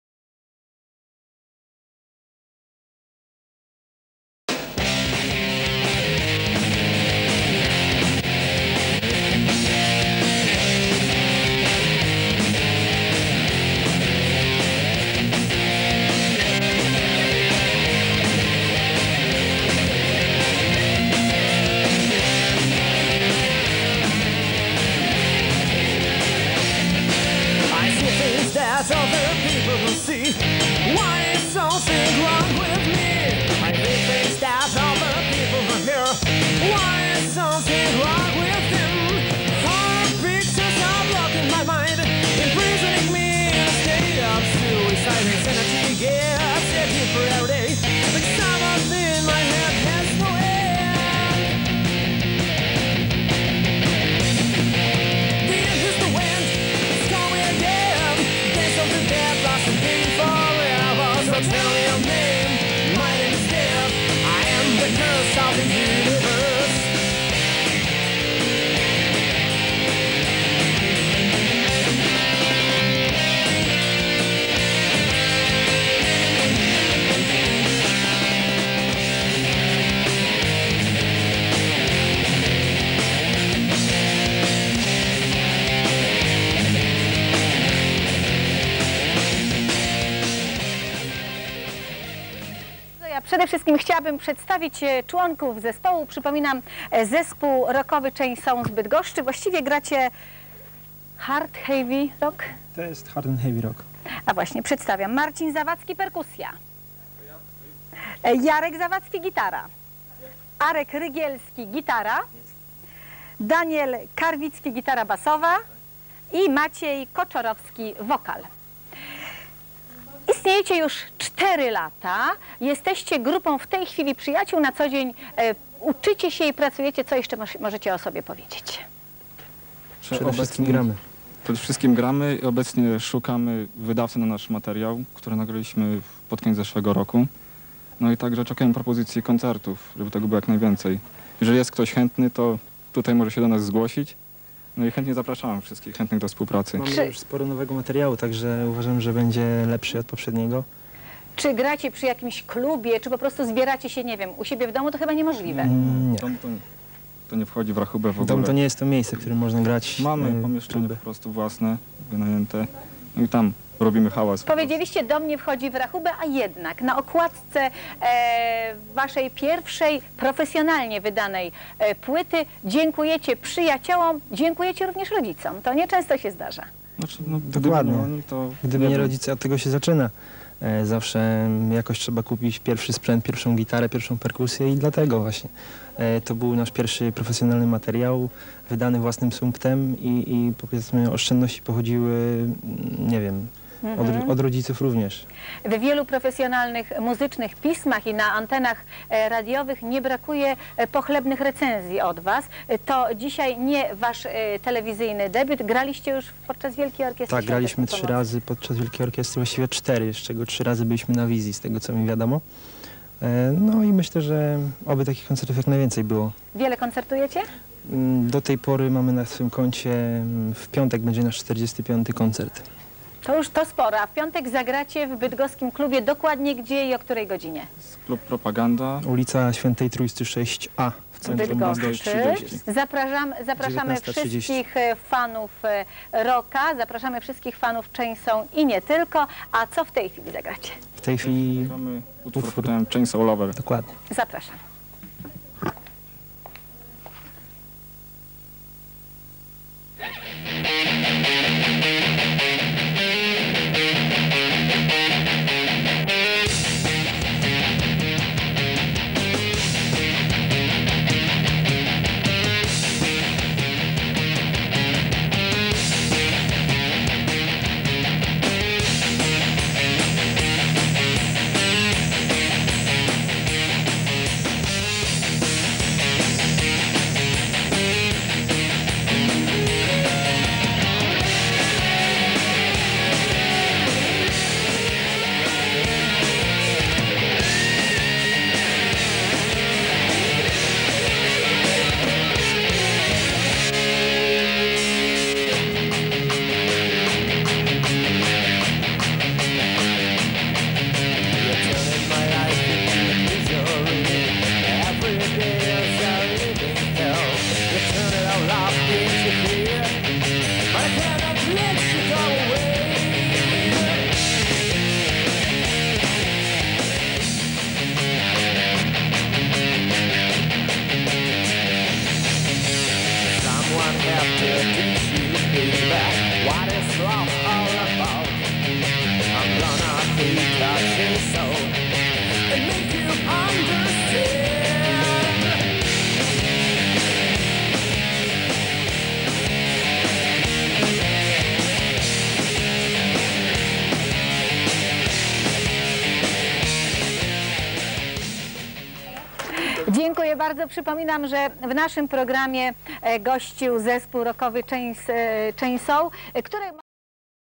Chainsaw in TV Bydgoszcz 2001
występ Chainsaw w TV Bydgoszcz, z powodu braku możliwości zagrania na żywo potraktowaliśmy to z przymrużeniem  oka :)
playback :)